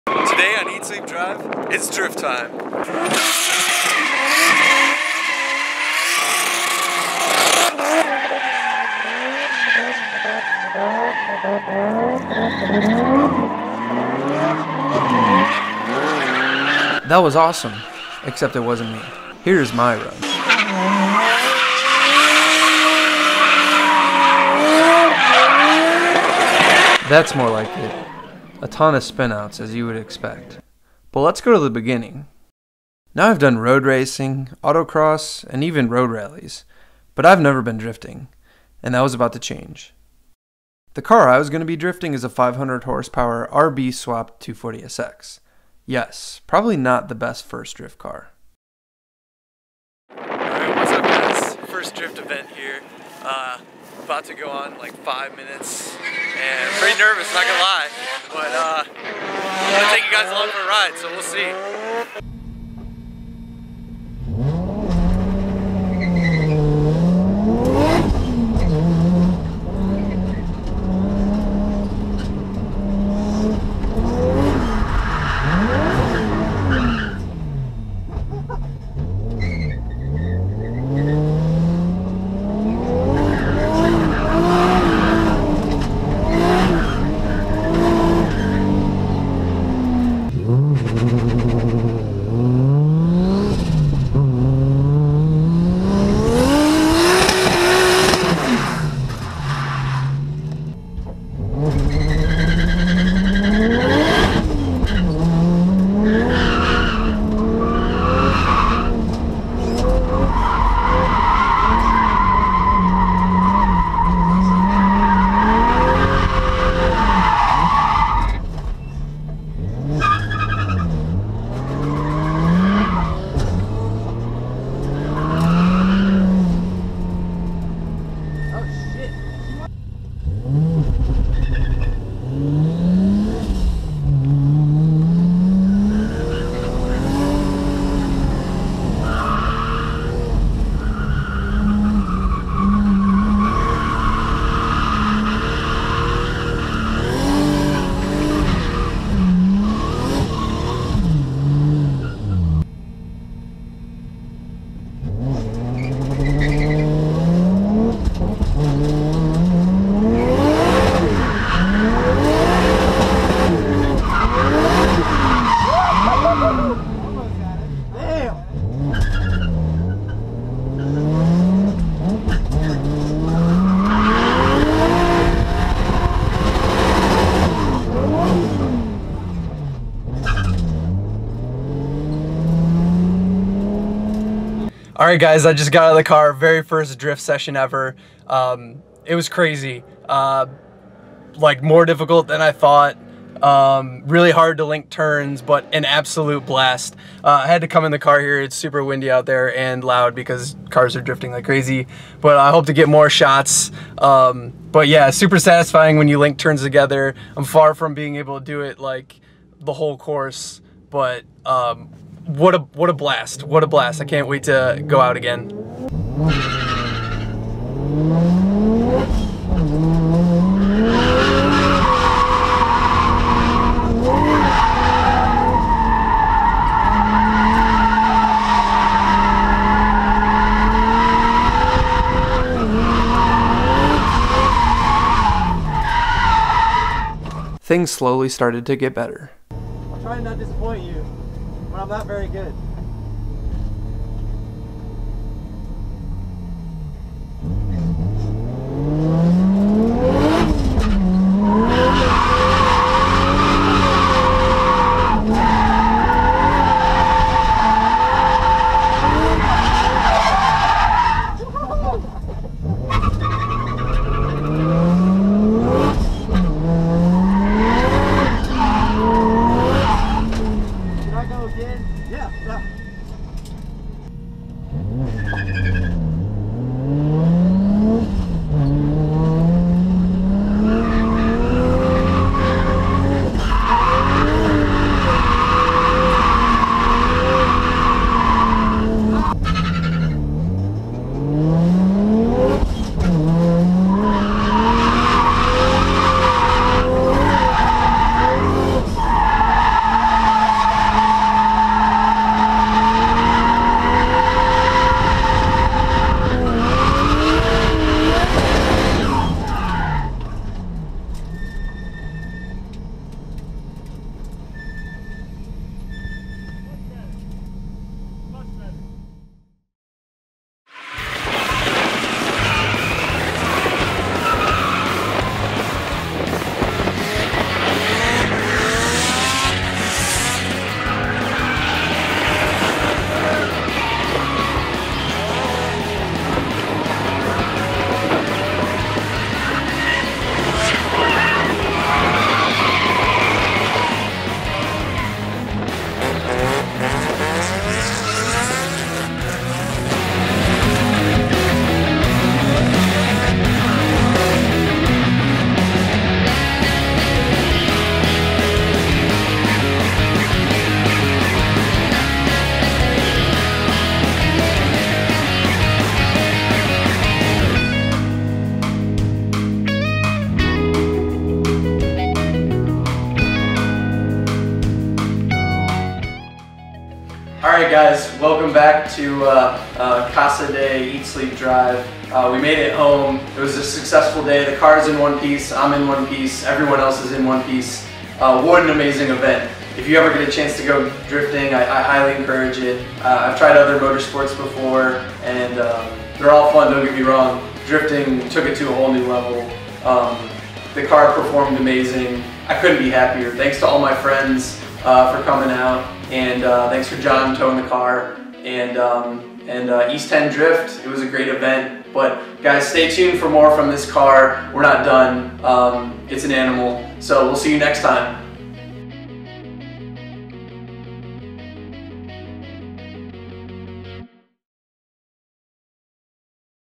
Learning to Drift - My First Time Drifting in a RB26 240sx
I've always wanted to try drifting and, since I just purchased this 240sx, I figured "Why the hell not?"  I have 0 drifting experience and this was my first event ever.  Completely clueless going in, you'll see what it's like to try drifting for the first time and how much fun it is regardless!